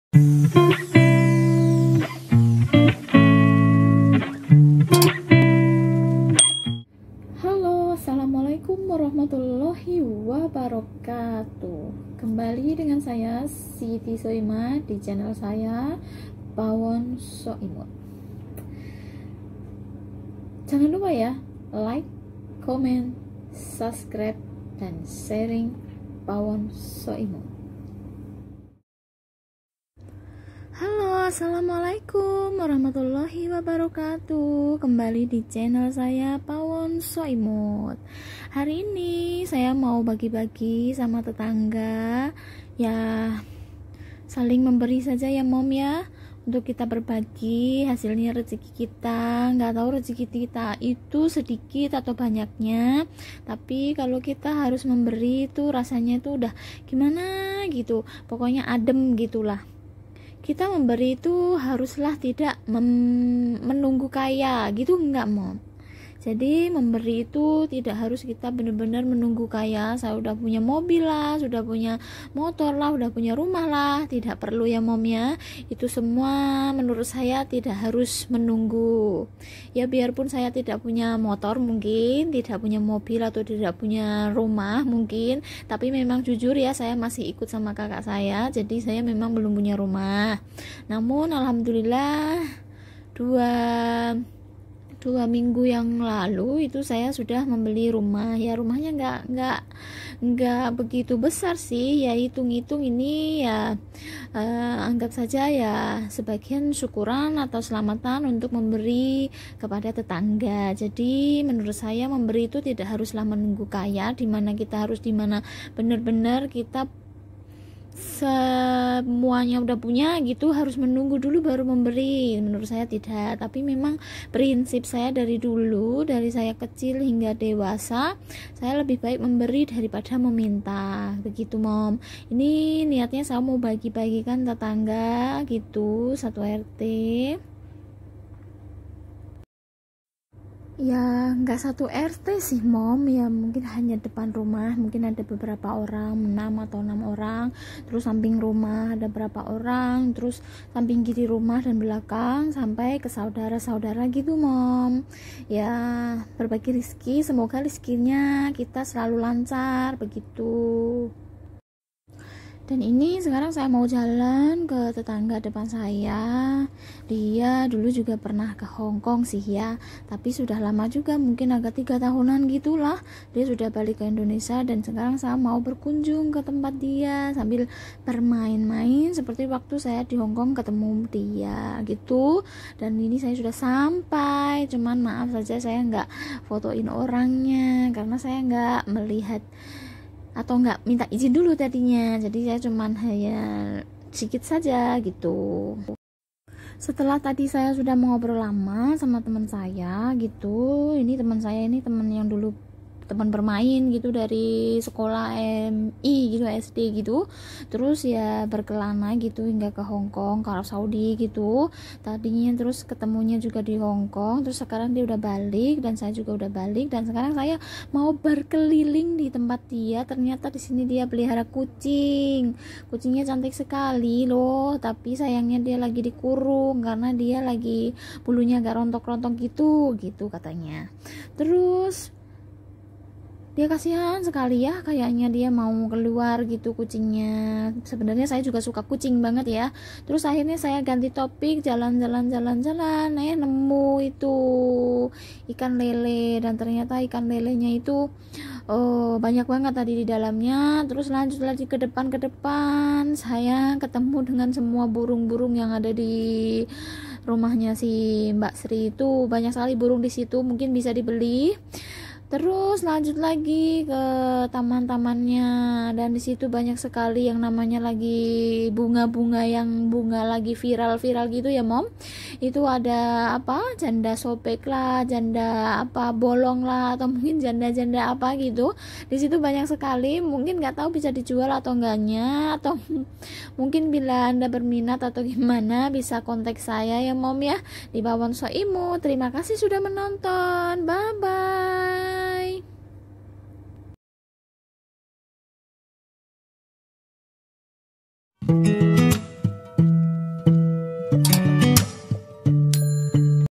JUM,AT BERKAH || BERBAGI || PAWON SOIMUT pawonsoimut berbagi sitikiranaolshop tkwhongkong .
hai mom ..
jum.at ini saya mau bagi bagi ..iya ngak banyak sih ..tp alhamdhulillah ..
biar .
dgn berbagi rizki kita slalu lancar aamiin
yarobbalalaamiin.
by pawon soimut